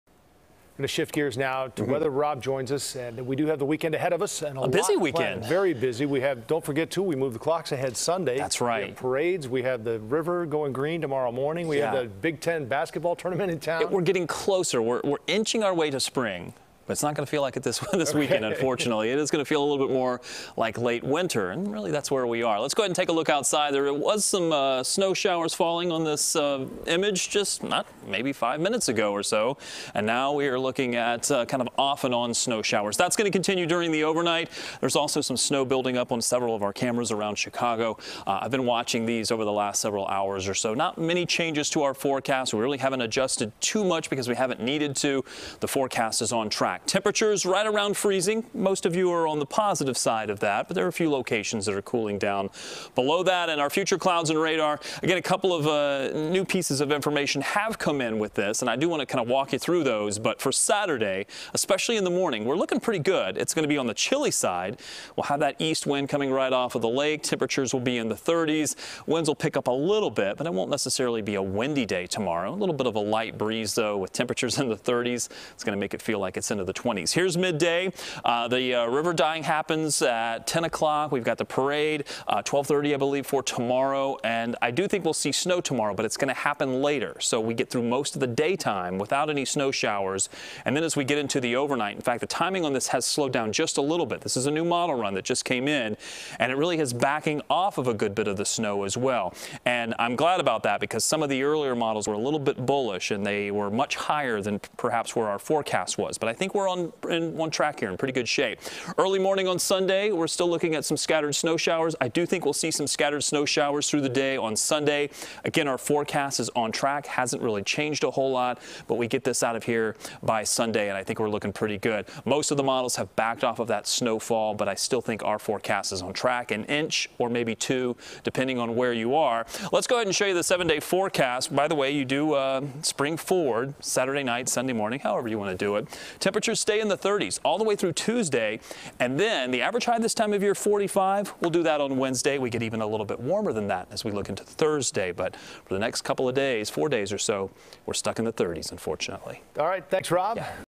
Chicago First Alert Weather: More snow this weekend
CBS 2 Meteorologist Robb Ellis has your 10 p.m. First Alert Weather forecast for Friday, March 10, 2023.